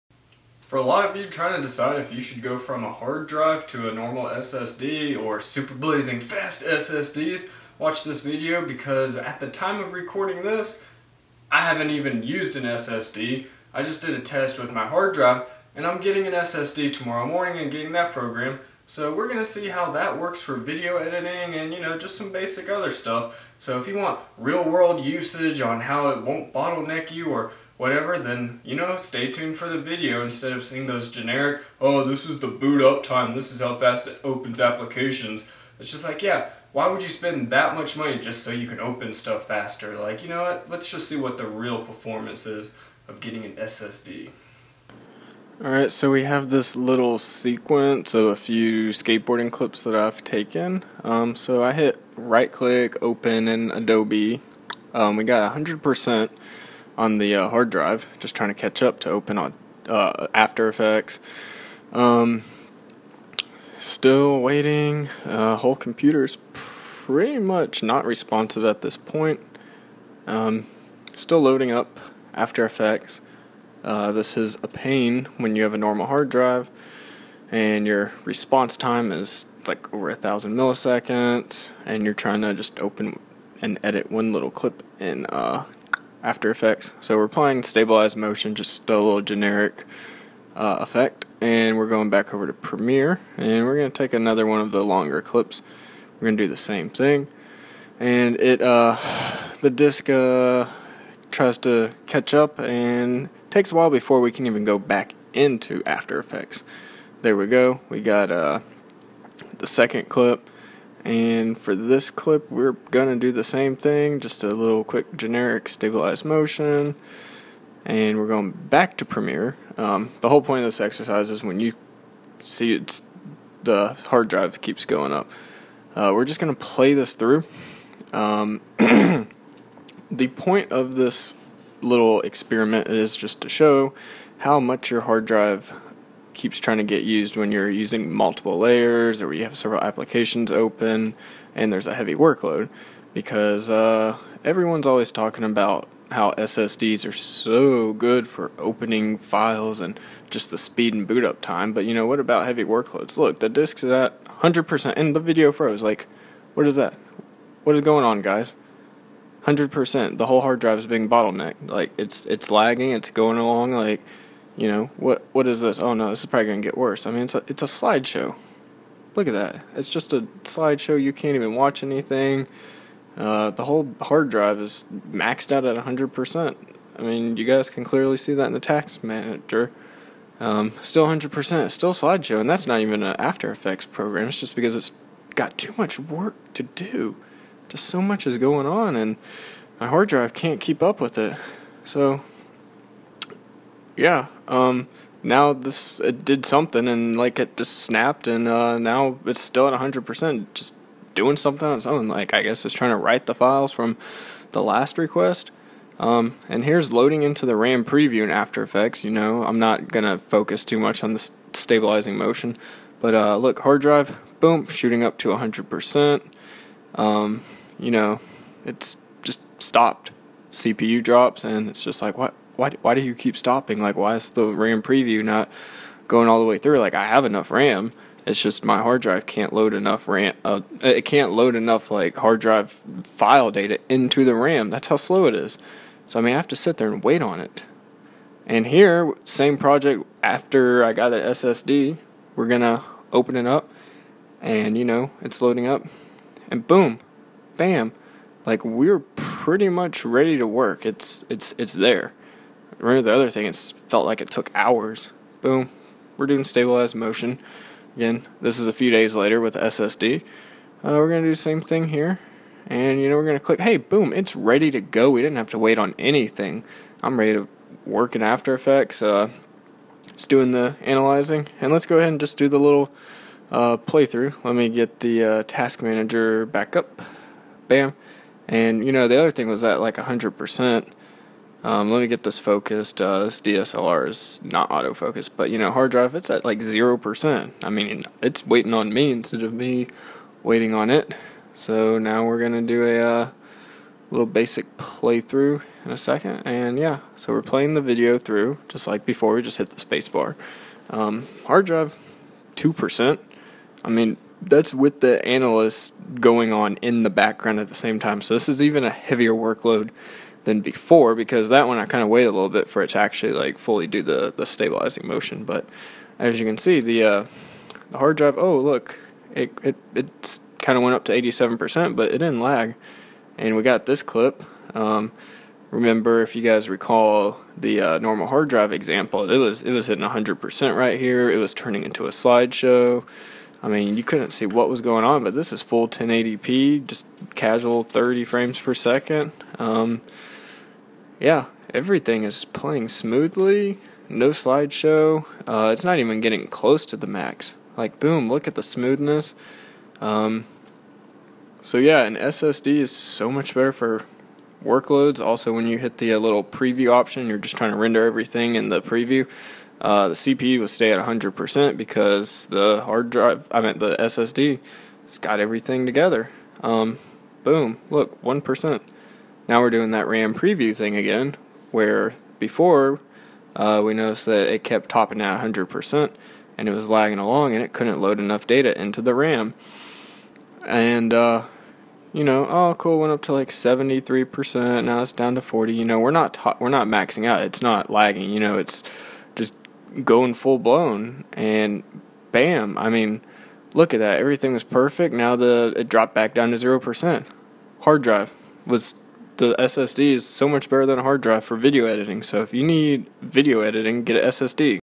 Why an SSD is a MUST for Video Editors!
A lot of video editors really argue against switching from a normal Hard Drive to a Solid State Drive because SSDs don't hold as much storage and are more expensive. Yes, you can store more video files on a HDD, but an SSD can help you produce content much faster and seamlessly. I got the Sandisk SSD Plus entry level drive. It's not even as fast as the other SSDs and PCI-E SSDs on the market, but even a cheap SSD will blow a normal hard drive away. I did two identical video editing tests just switching back and forth between Adobe Premiere Pro and After Effects and a playthrough with RAM Preview. This is a real world example of an SSD. Not just benchmarks. You have to see it to believe it. Obviously doing video editing with larger projects, higher resolutions, more layers and effects, the SSD will pull ahead even more past the HDD.